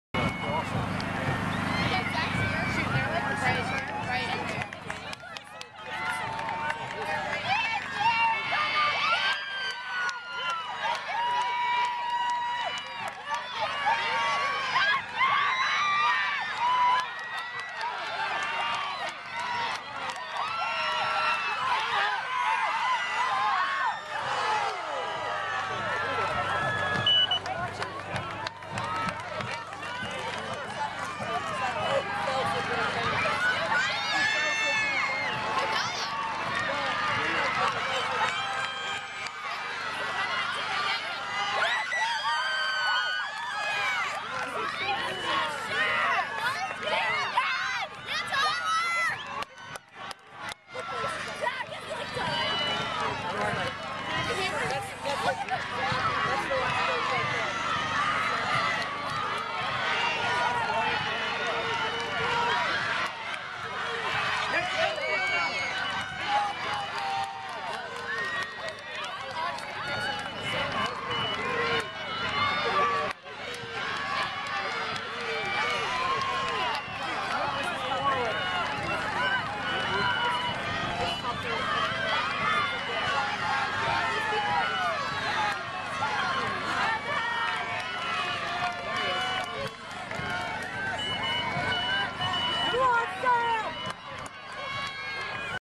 L-L boys' cross country finals
Action from the finish line at the L-L League boys' cross country championship, won by Lancaster Mennonite's Jared Schatz on 10-18-11.